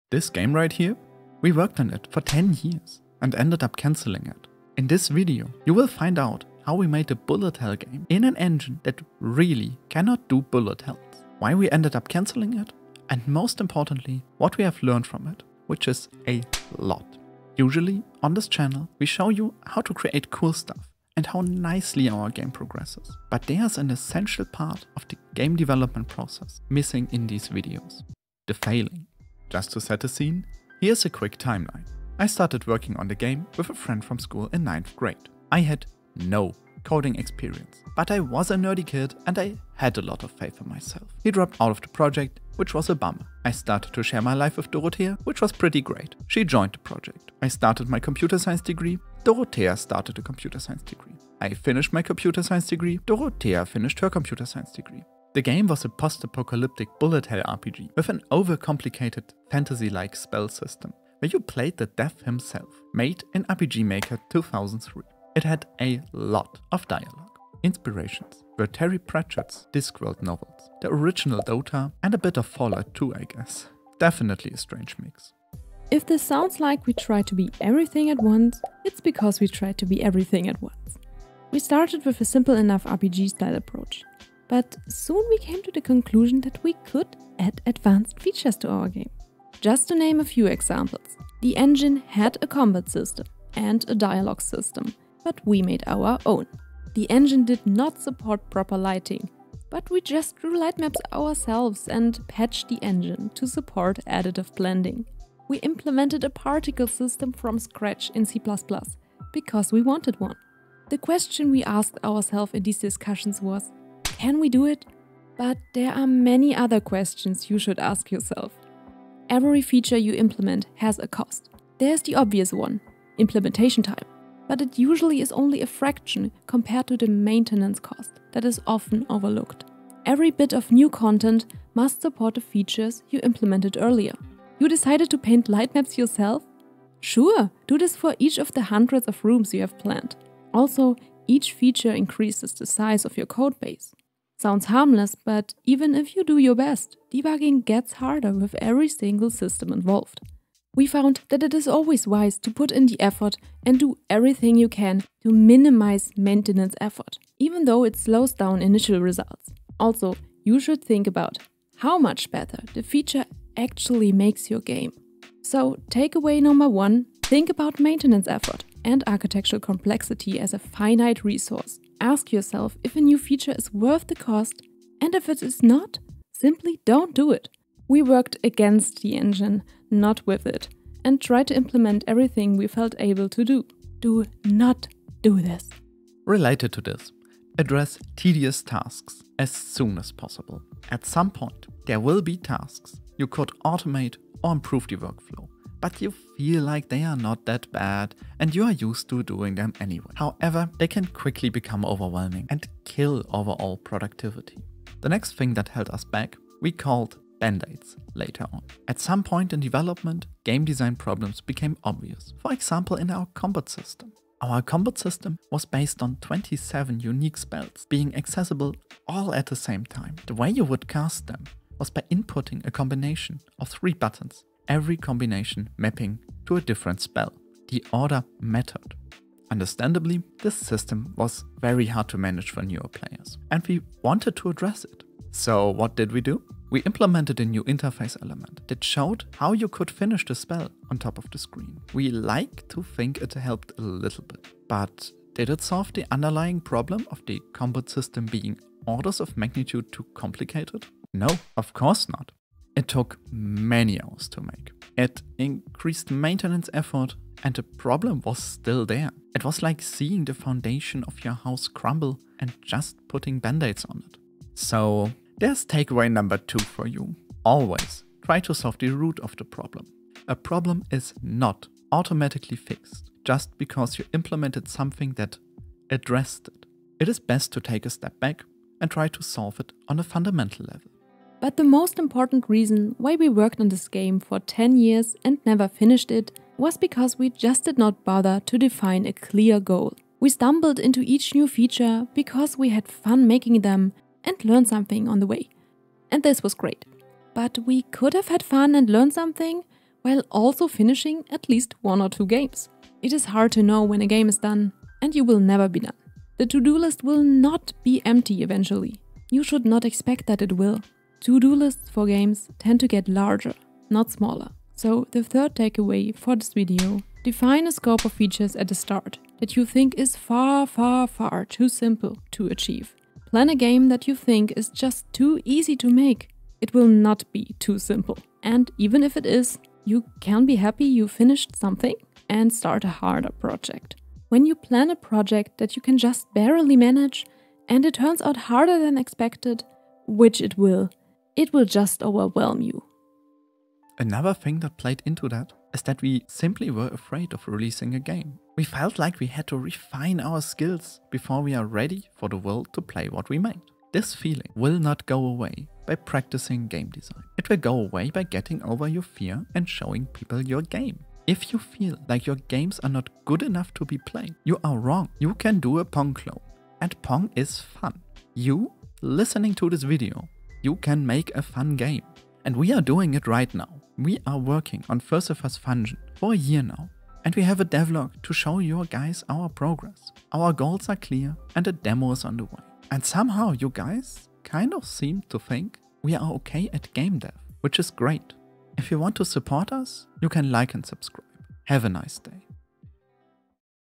I Worked 10 YEARS On This Game, Then CANCELLED It.
People ask how we learned game dev. It was the hard way.
But you can learn from our mistakes!